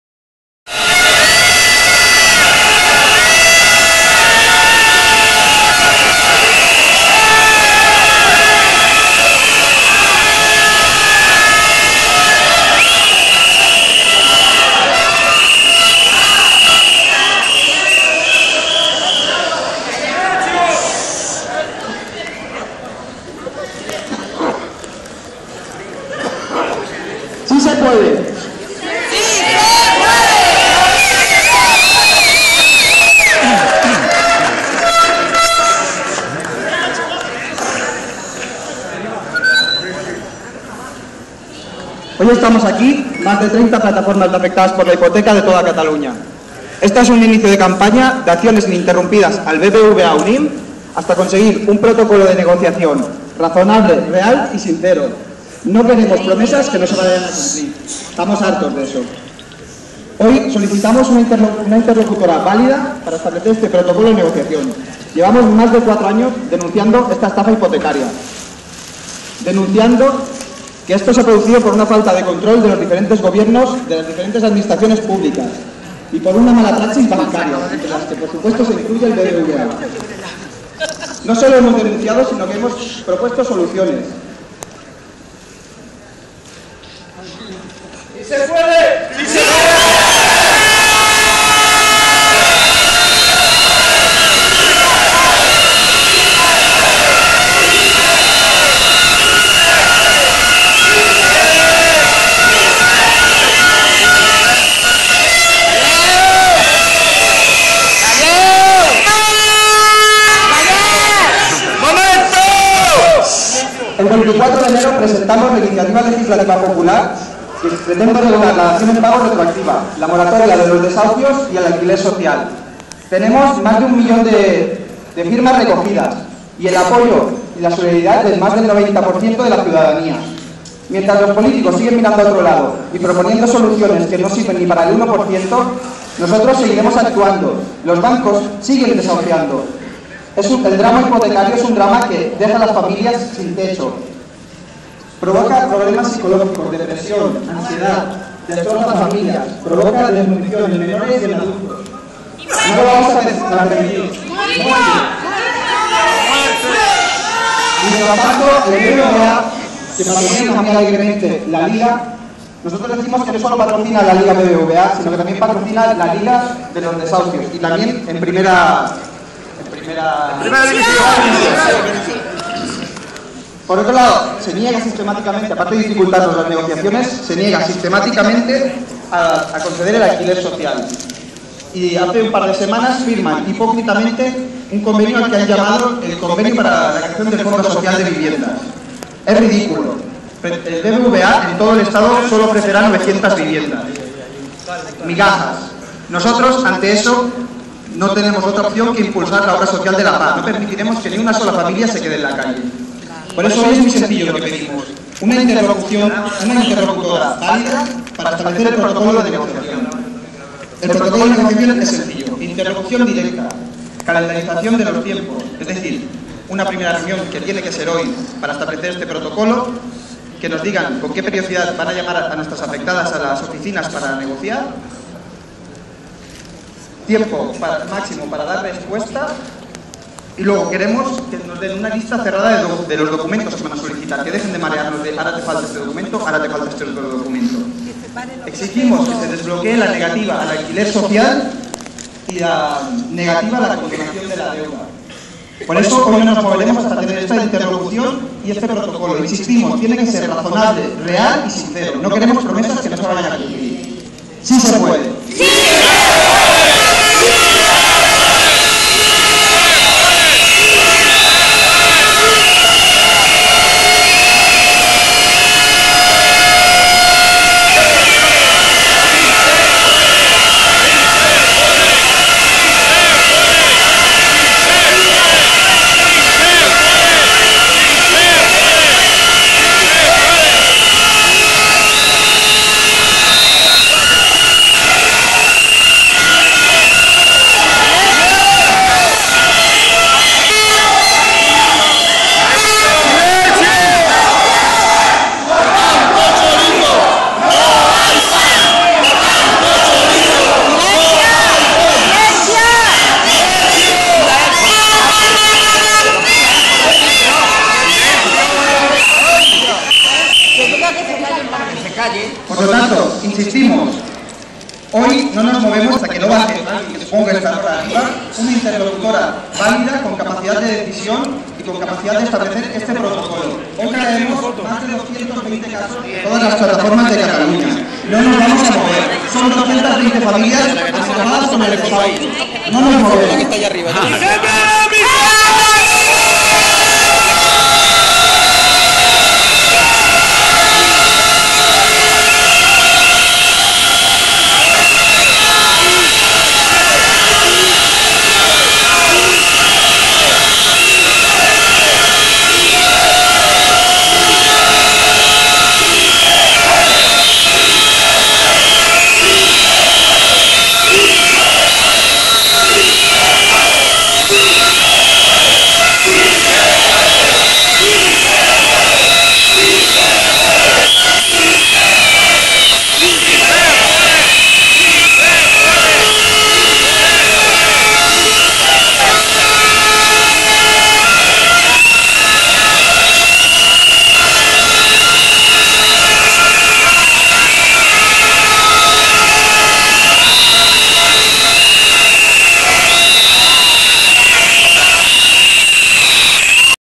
Les PAH Okupen BBVA part 2 de 40
El dia 7 de Enero , las PAH de toda Catalunya hicimos una acción organizada okupando la sede del BBVA de Barcelona , situada en Plaça Catalunya , para conseguir un canal de negociación directa y poder encontrar soluciones a todos los casos que la entidad BBVA-Unnim tenia bloqueados.
El dia 7 de Febrer de 2013 ,, les PAH de tota Catalunya vam fer una acció organitzada okupant la seu del BBVA de Barcelona , situada a la Plaça Catalunya , per tal d'aconseguir un canal de negociació directa i poder solucionar els casos que l'entitat BBVA-Unnim tenia bloquejats.